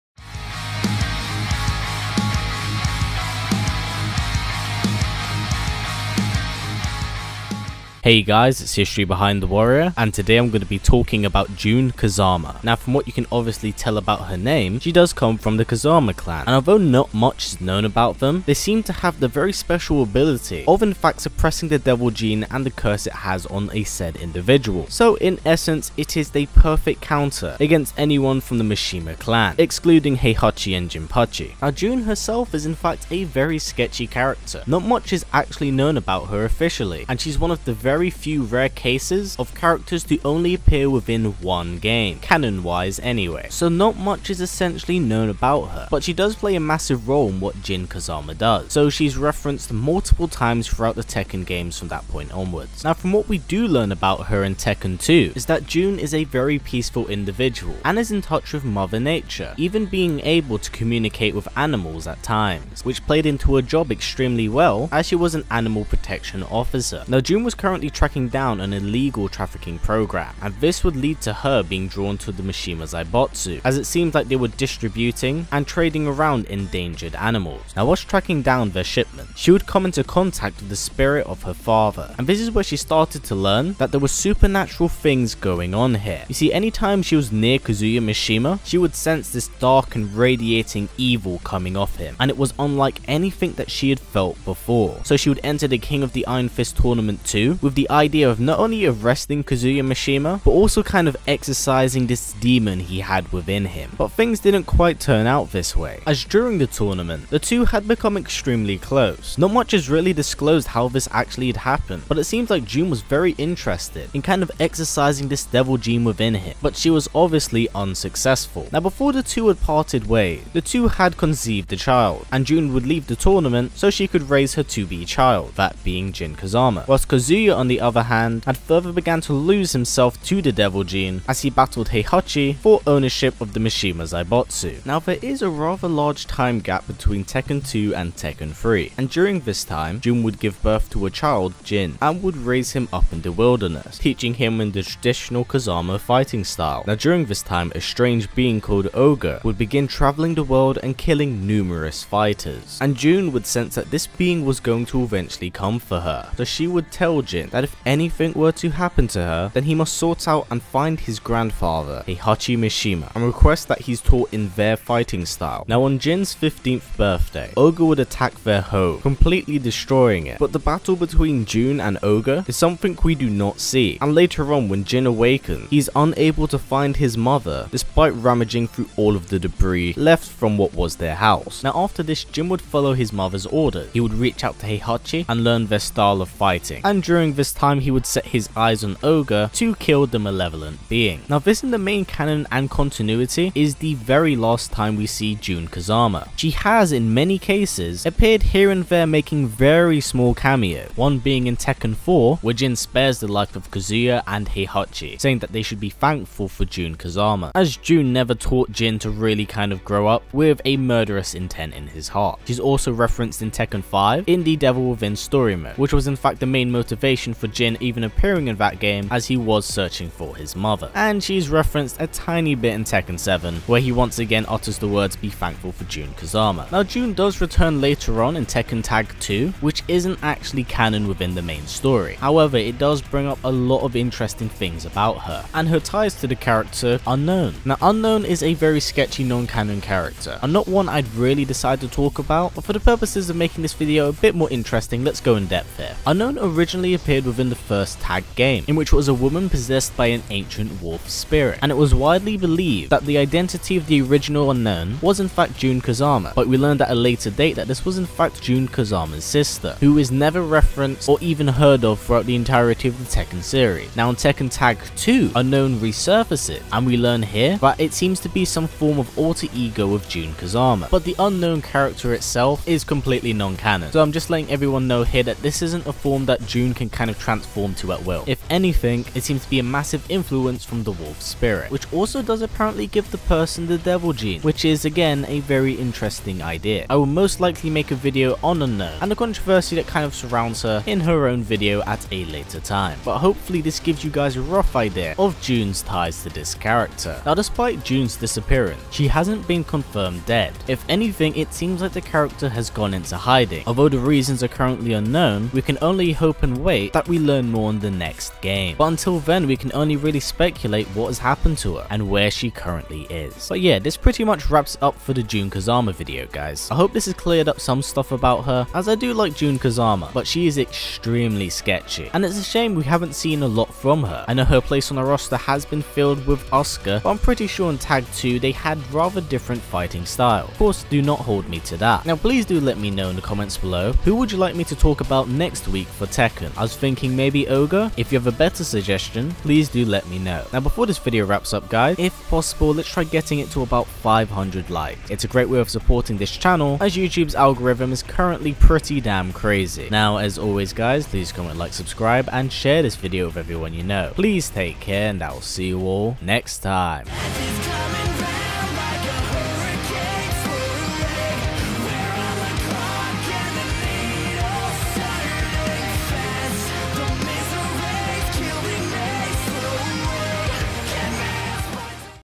History Of Jun Kazama Tekken 7
Hey guys!

Another Tekken video today and we're gonna be talking about Jun Kazama!
How do you feel about the character? Would you like to see her return? Or remain in the shadows?

Dont forget to let me know who you'd like to see next!
And feel free to get in touch with me here!